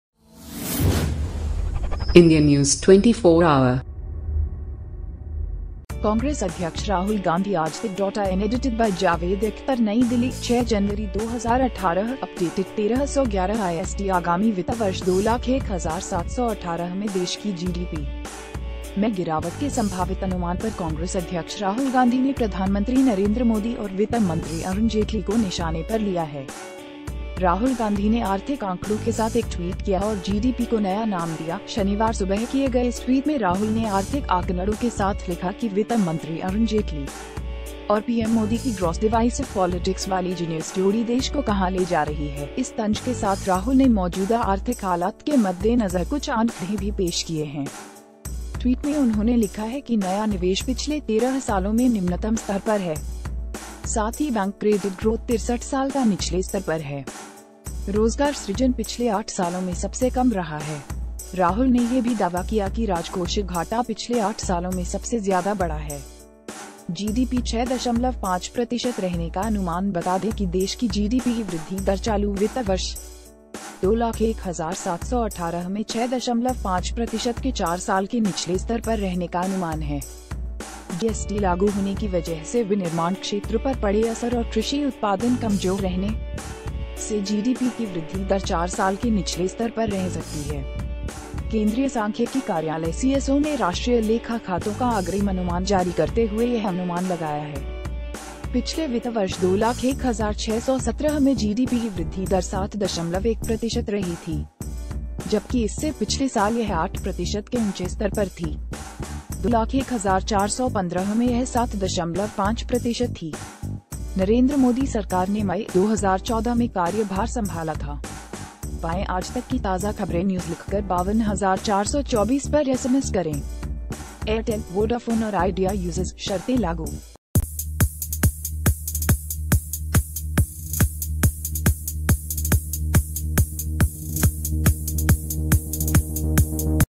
राहुल ने जीडीपी को बताया 'सकल विभाजनकारी राजनीति', पीएम मोदी-जटेली को भी घेरा
कांग्रेस अध्यक्ष राहुल गांधी aajtak.in Edited By जावेद अख़्तर नई दिल्ली, 06 जनवरी 2018, अपडेटेड 1311 IST आगामी वित्त वर्ष 201718 में देश की जीडीपी में गिरावट के संभावित अनुमान पर कांग्रेस अध्यक्ष राहुल गांधी ने प्रधानमंत्री नरेंद्र मोदी और वित्त मंत्...